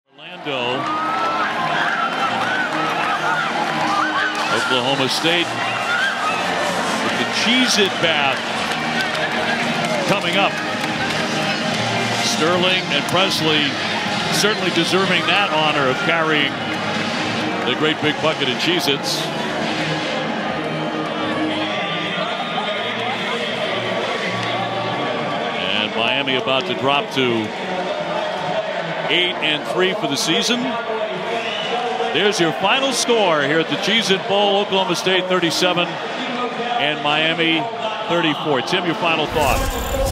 Oklahoma State "Cheez-It Bath" Celebration vs Miami | 2020 College Football
Oklahoma State celebrates their 2020 Cheez-It Bowl victory over Miami by giving their coach Mike Gundy a Cheez-it bath. With the 37-34 win, the Oklahoma State Cowboys finish the season 8-3, while the Hurricanes drop from 8-2 to 8-3.